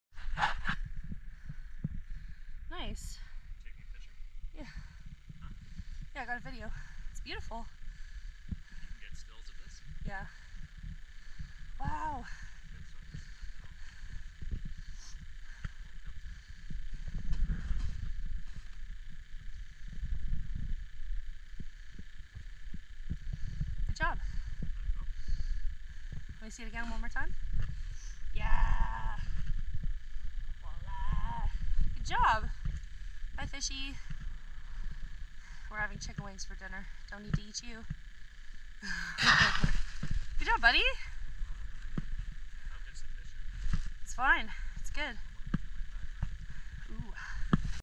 Ice fishing in the Kawartha's
Fishing for perch but caught a walleye released it nicely.